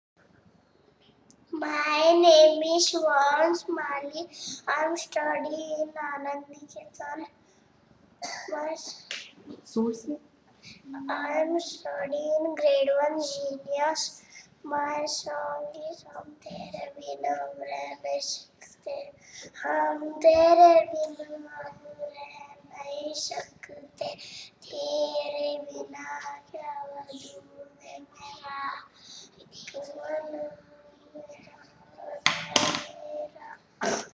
Vansh Mali - Anand Niketan (1st Standard)
Vansh Mali - Anand Niketan (1st Standard) Is Singing a Song For Indian Idol Academy, Festival Of Music, Presented By Havmor.